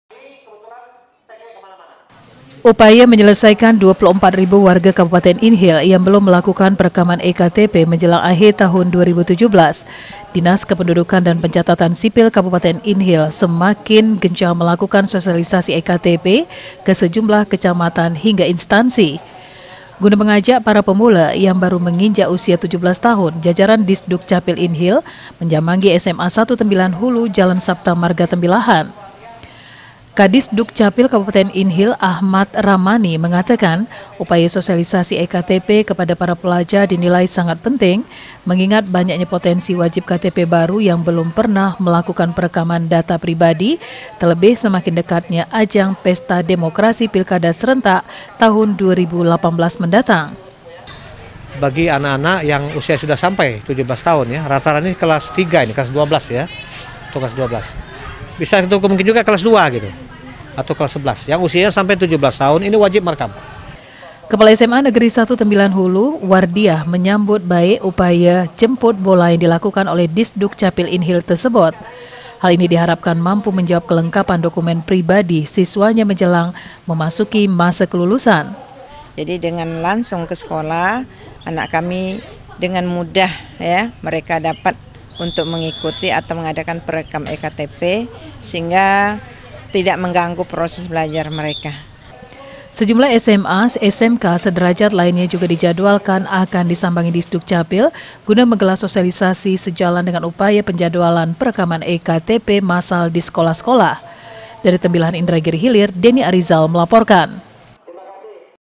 Disdukcapail Inhil Gelar Sosialisasi E-KTP untuk Siswa
Guna mengejar target 24 ribu perekaman E-KTP hingga Desember 2017. Disdukcapil Inhil menggelar Sosialisasi E-KTP kepada ratusan Pelajar di SMA 1 Tambilahan Hulu.